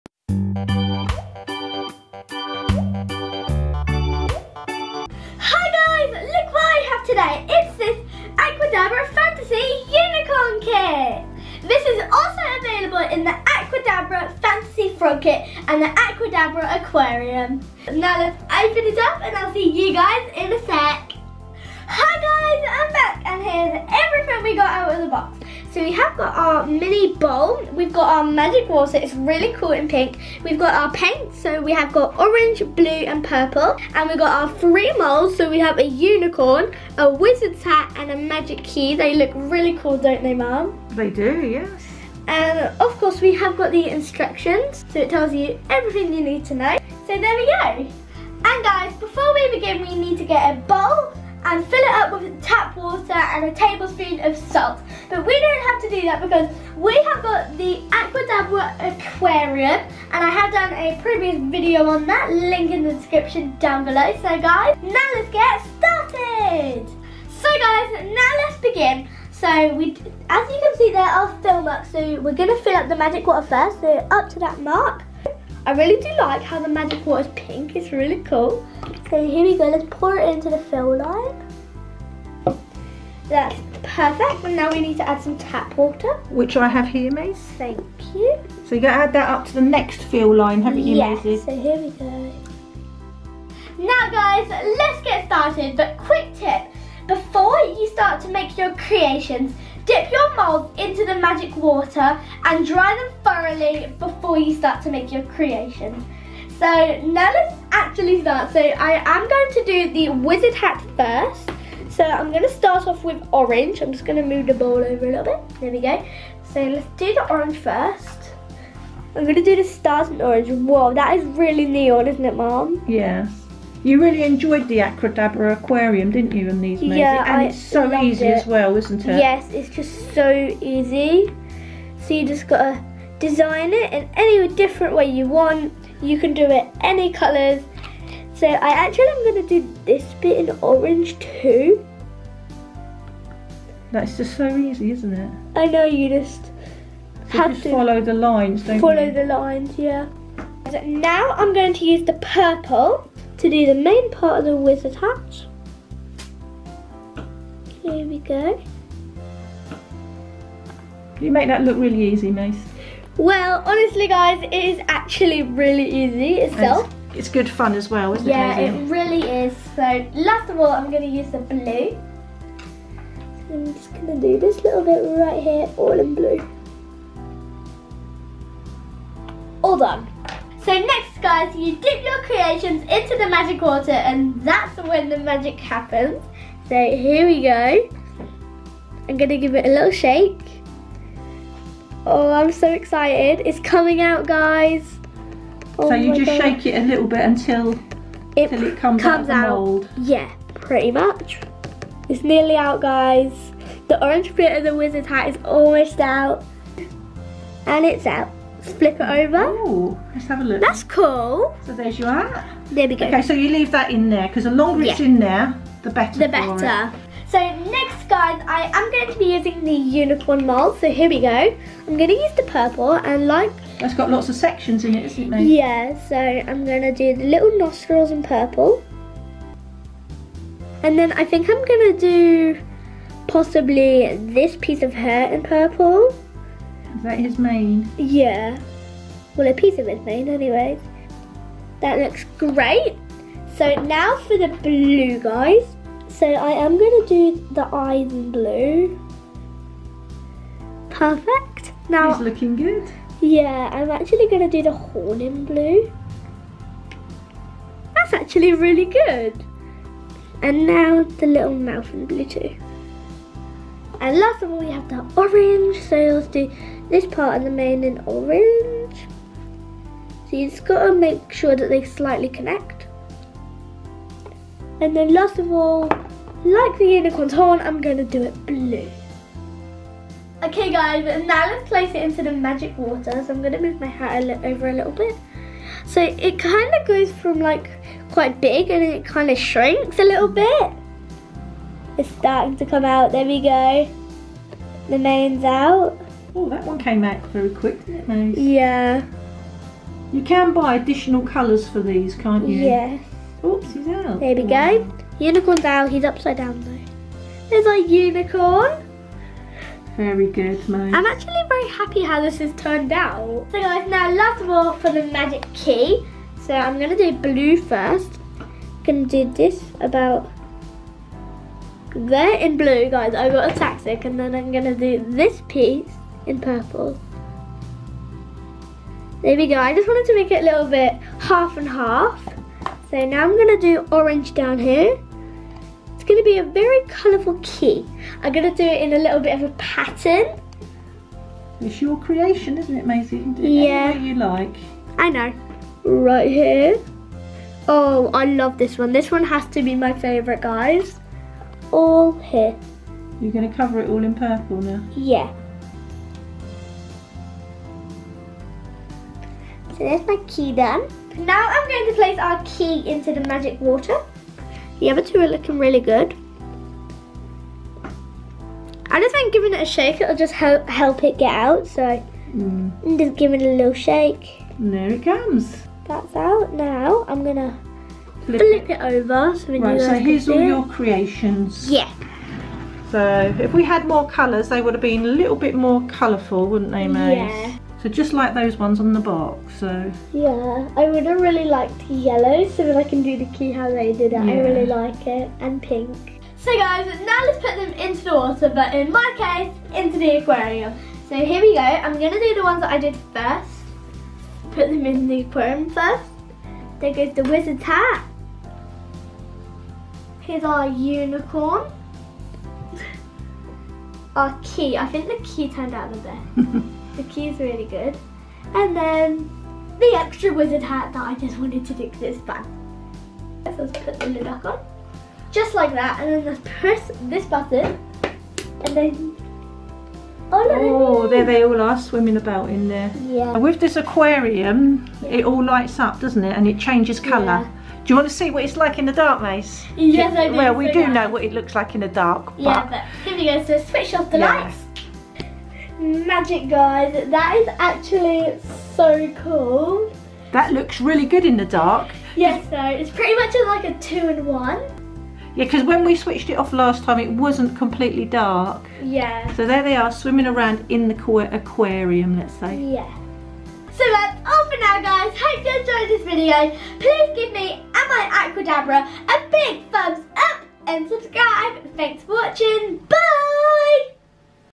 AQUADABRA FANTASY KIT
I really enjoyed this Aquadabra fantasy kit it's so easy just add your creations to the magic solution and tap water and see the magic begin!  I also made a video on the Aquadabra Aqurium which inspired me to try out this fantasy kit.